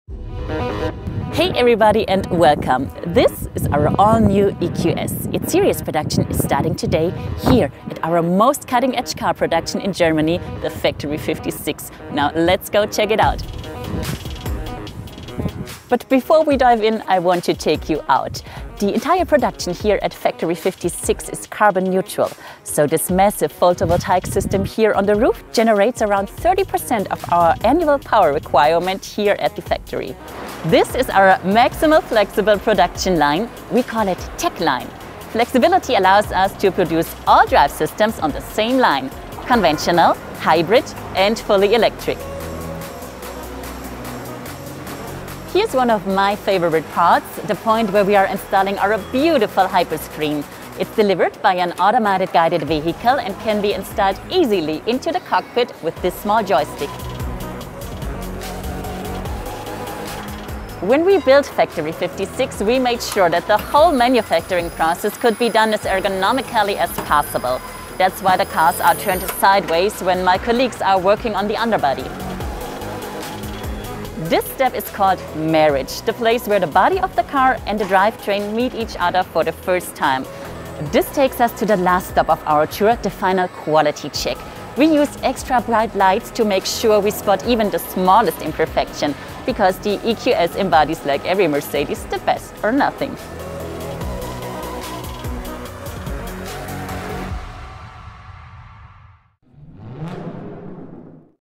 Start of production of the Electric Mercedes EQS in the Factory 56
A new definition of luxury meets the future of production at Mercedes-Benz: production of the EQS electric saloon started at Factory 56 in the Mercedes-Benz Sindelfingen plant - flexible, digital, efficient and sustainable. The EQS is integrated into ongoing series production at the site as the first all-electric model. The Mercedes-Benz S-Class saloon, the long-wheelbase version and the Mercedes-Maybach S-Class are already rolling off the production line at Factory 56.